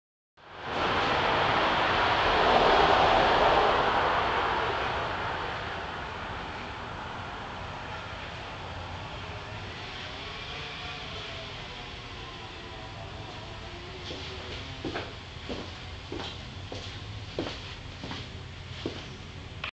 A-C axis head manual mock-up
rotational motion test with A & C axes.